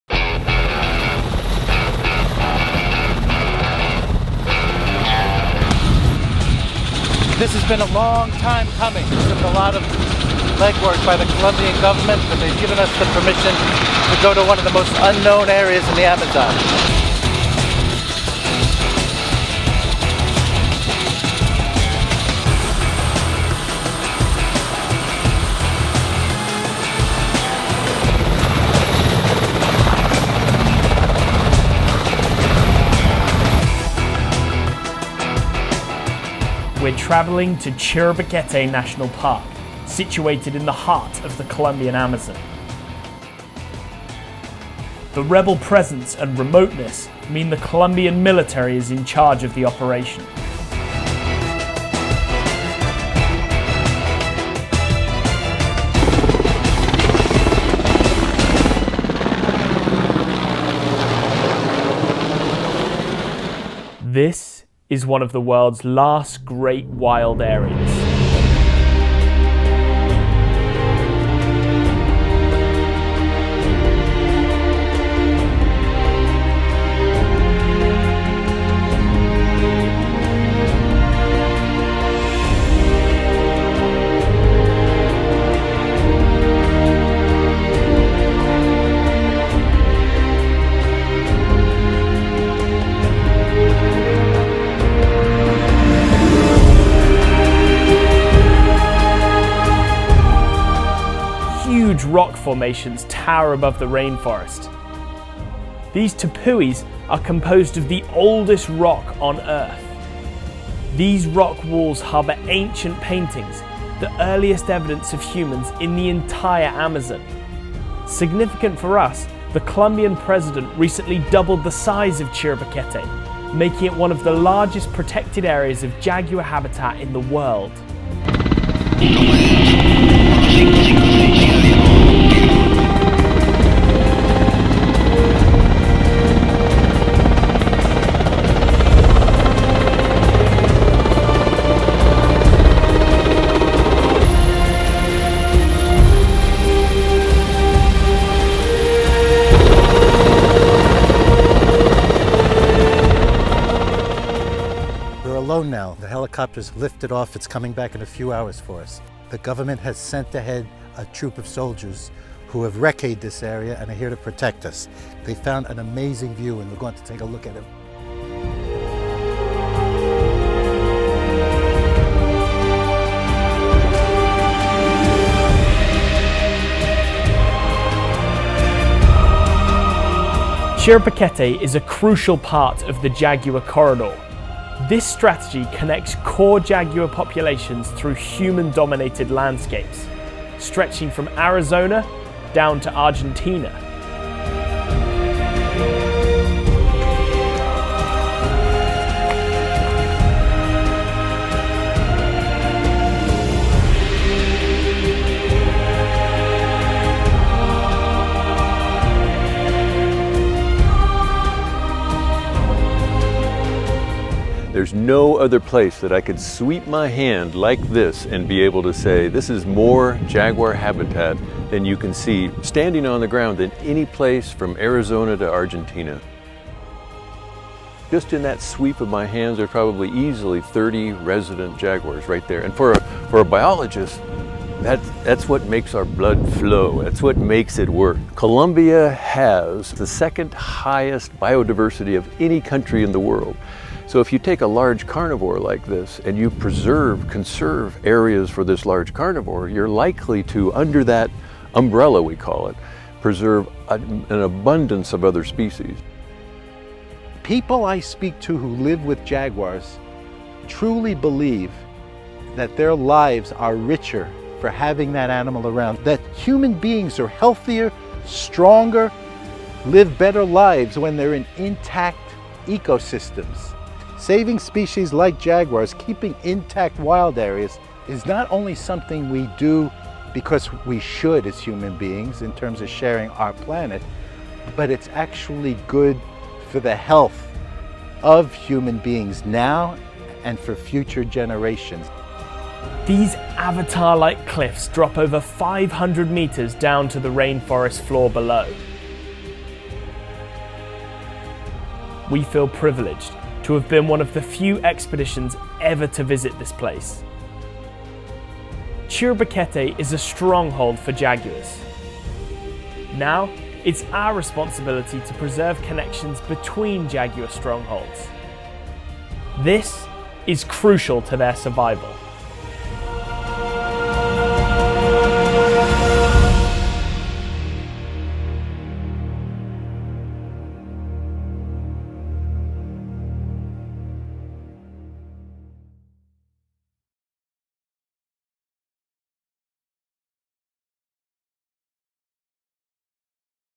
Full Colombia Film
Dr. Alan Rabinowitz, Panthera CEO, visits the Colombia Amazon to help preserve one of the largest natural jaguar habitats in the world.